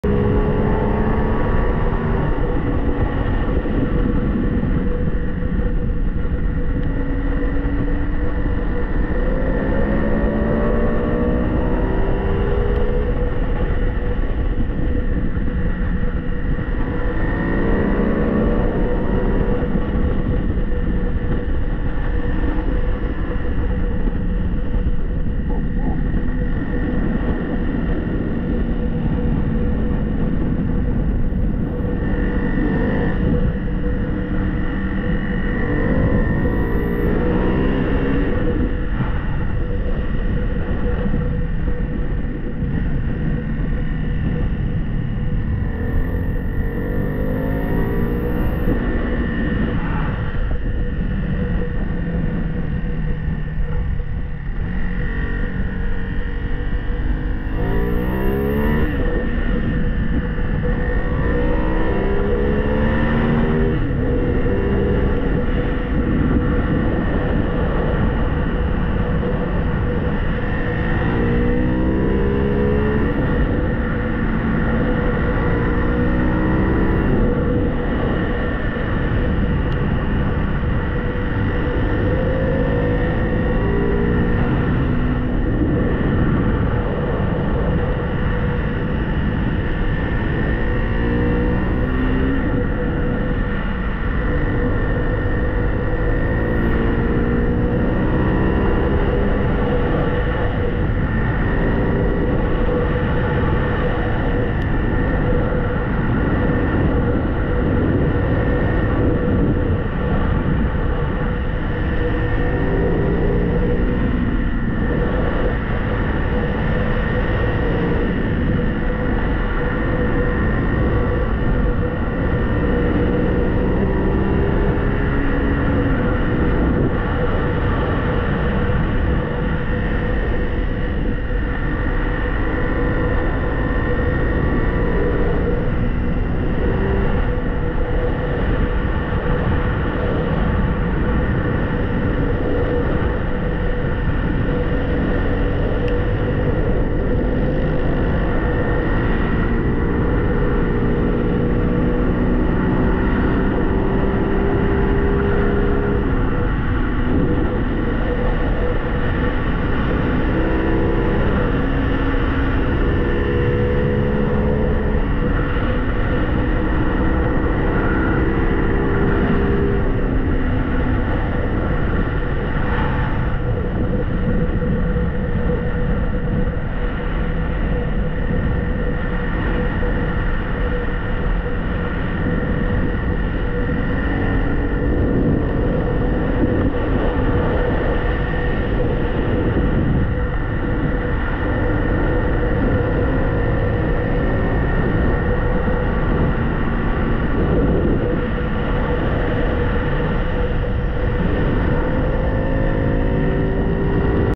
Ducati Hypermotard 698 Mono on RT 94 with Sully
riding the Ducat Hypermotard 698 Mono on RT 94 towards Campo, getting stuck behind migrants an Border Patrol clogging the feed